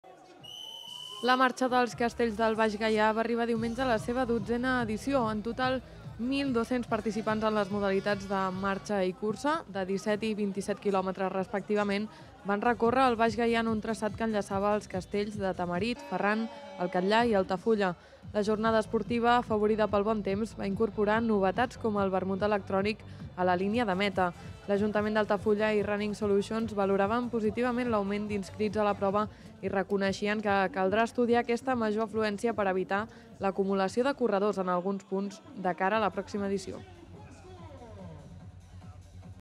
La Marxa dels Castells del Baix Gaià aplega 1.200 corredors
Caminants i corredors de les modalitats de marxa i cursa van completar el circuit que enllaçava els castells de Tamarit, Ferran, El Catllar i Altafulla